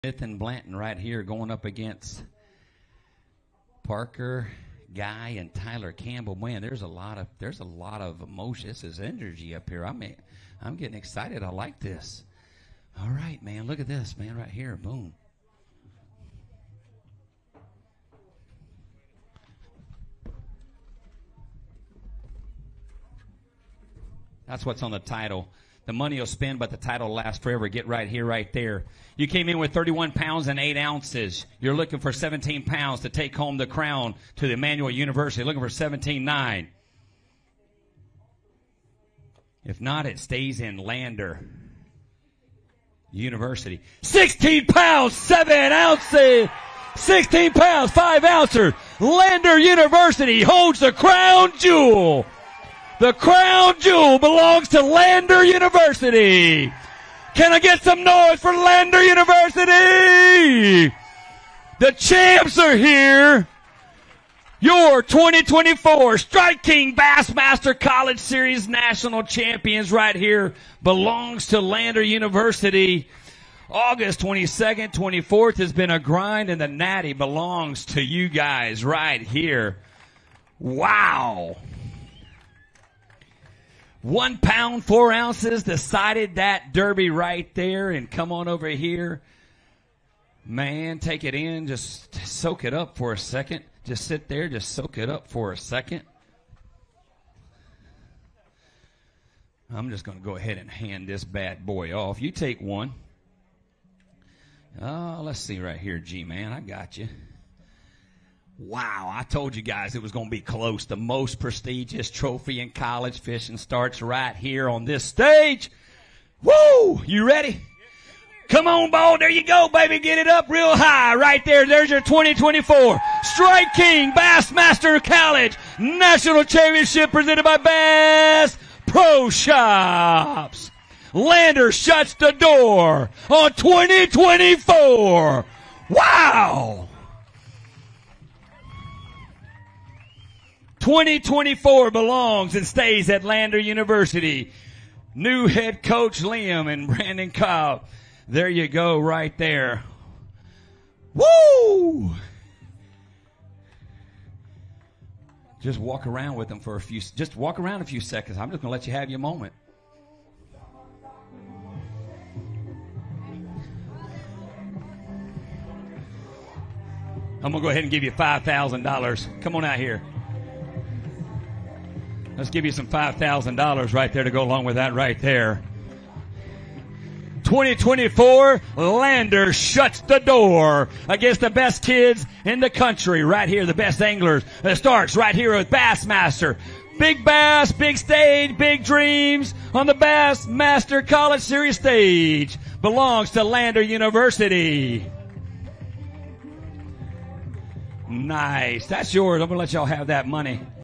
Lander University’s Smith and Blanton win 2024 Bassmaster College Championship
Garrett Smith and Andrew Blanton of Lander University are the 2024 Strike King Bassmaster College Series National Champions. The duo won the three-day Championship on Lake Hartwell with 49 pounds.